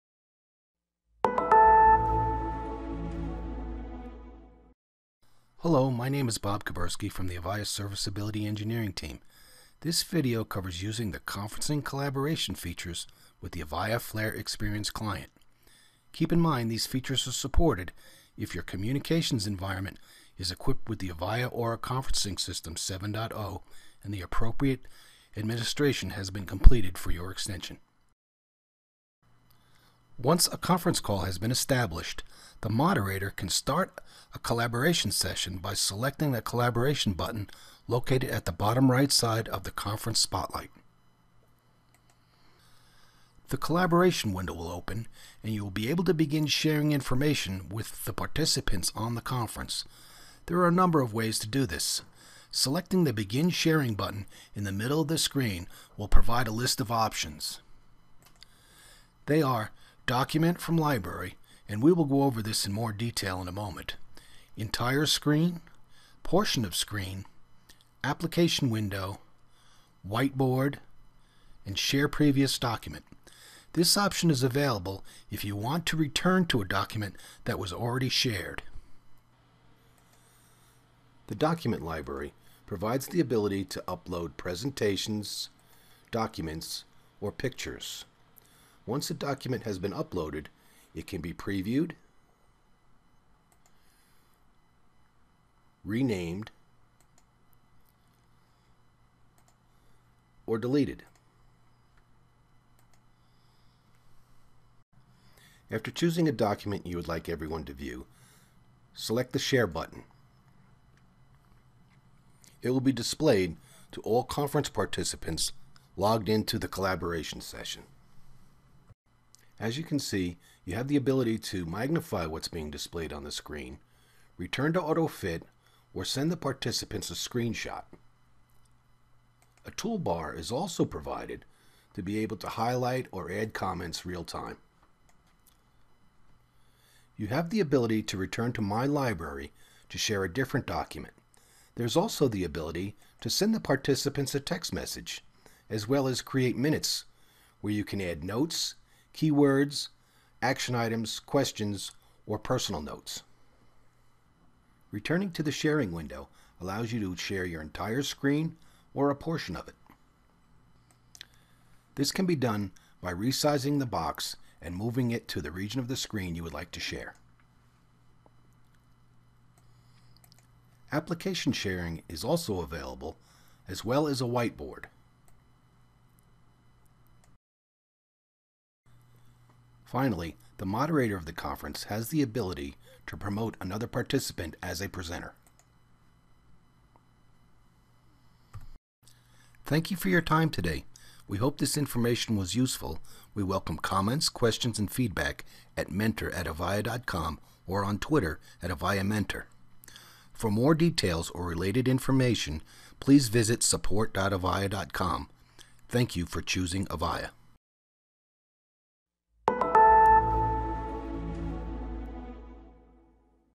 Avaya Flare Experience Conference Collaboration
An overview how to use the collaboration features with the Avaya Flare Experience client. Includes information on how to manage the document liberary. Produced by Bob Kuberski.